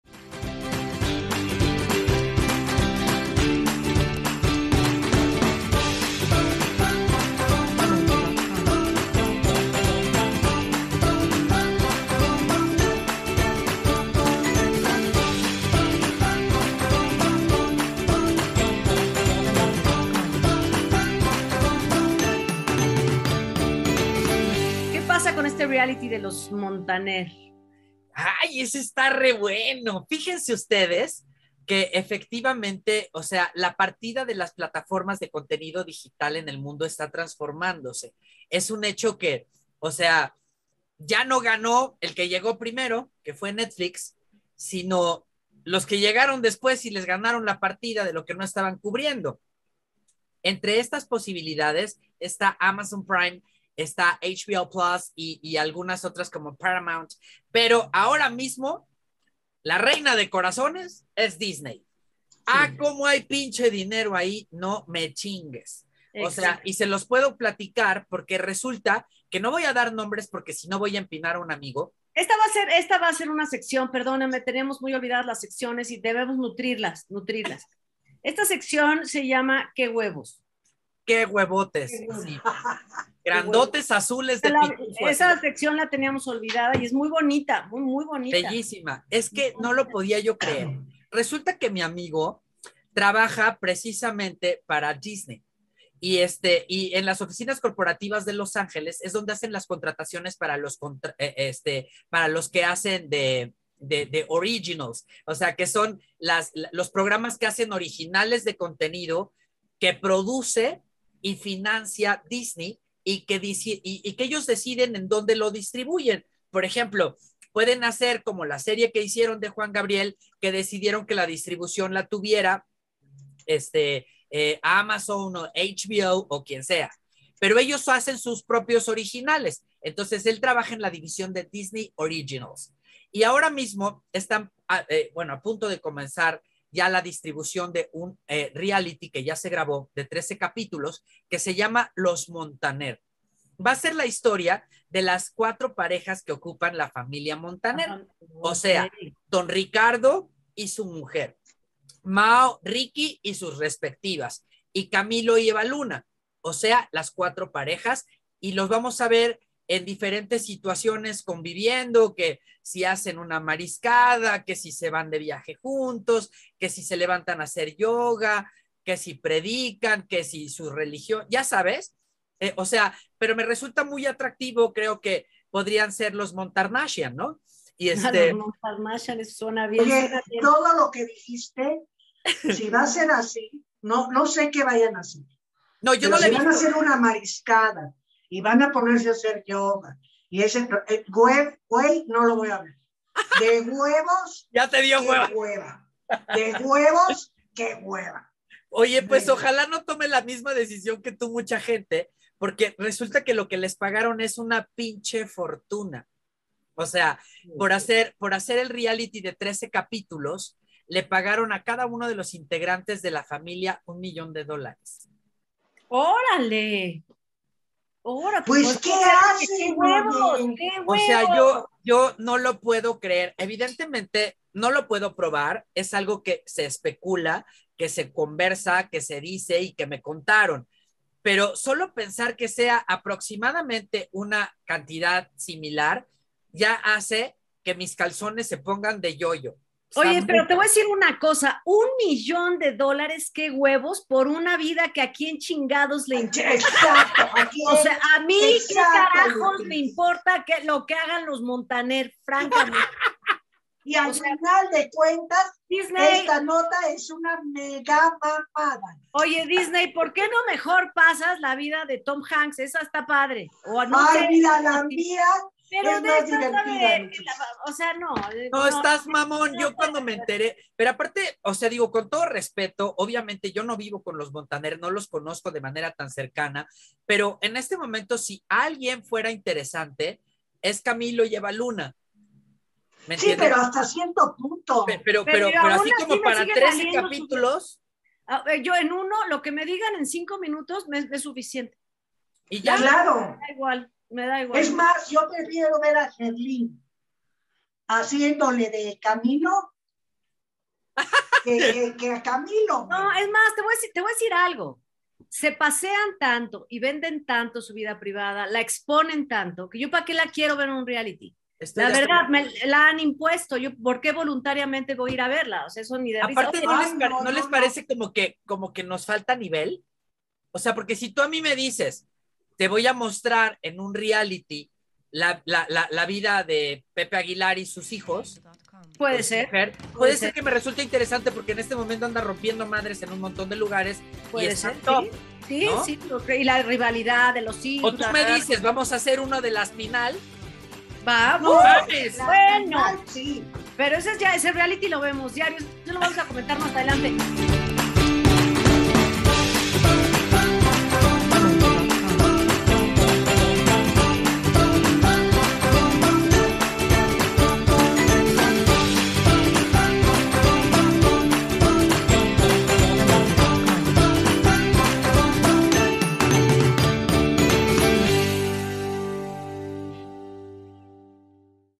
Carísimo reality de los Montaner ¿quién lo quiere ver?
programa de opinión sobre las notas más destacadas del entretenimiento y la vida en general. con Claudia de Icaza, Lupita Martínez, Luis Magaña, Poncho Martínez y Aurora Valle.